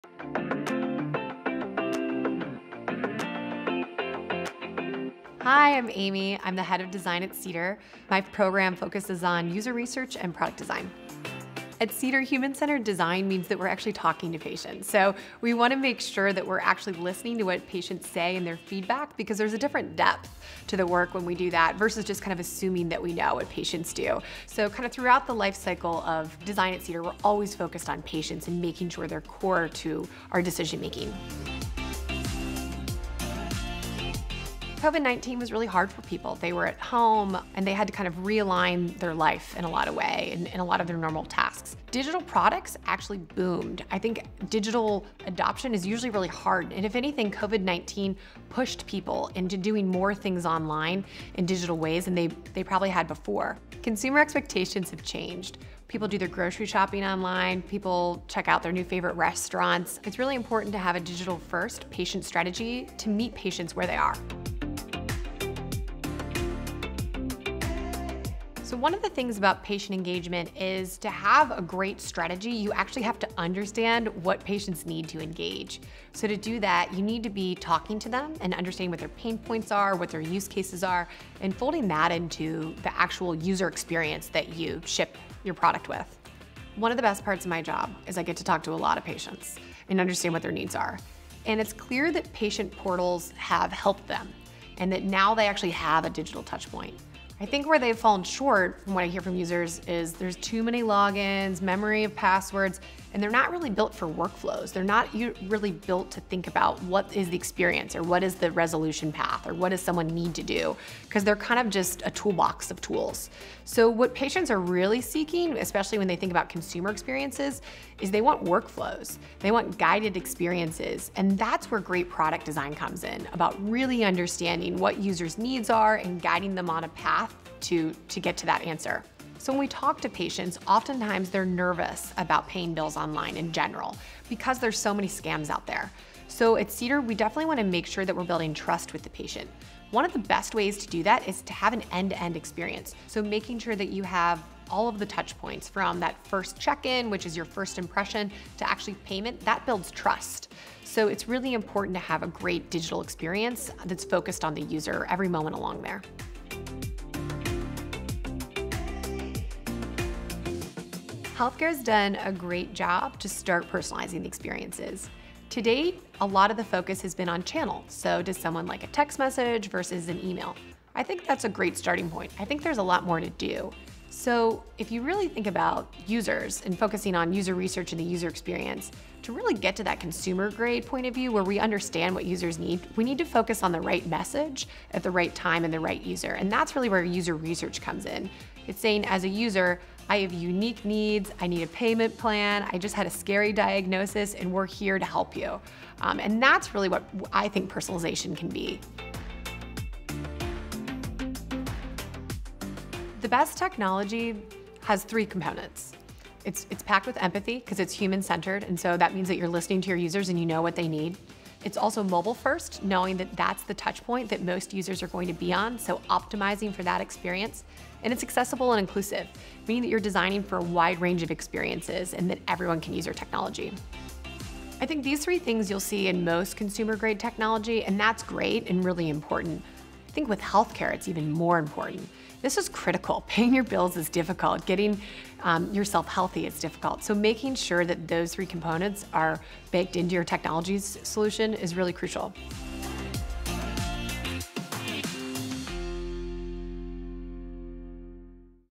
Personalizing the patient experience using Cedar's healthcare financial engagement platform
Amy Stillman, head of design at Cedar, talks about building a better patient engagement that respects their needs, their concerns and that delivers optimal workflows that are customized with information that patients need at the right time in the right way that is simple, easy to act on and that create a delightful experience just like the consumer experiences that we have become accustomed to with our ride sharing or e-commerce applications.

Core to the mission of building a world-class patient experience are these tenants: 1) Build simple, intuitive and relevant solutions for patients to pay their bills. 2) Deliver solutions for providers to collect payment for work they have done. 3) Minimize administrative burden for providers and payers to help them achieve better financial results.

Delivering on these things creates good for patients and good for providers. At Cedar, we foster that mutually beneficial relationship through proven expertise and modern technology. We combine the best techniques of fintech, ad tech, consumer and healthcare to help providers understand and engage patients more effectively.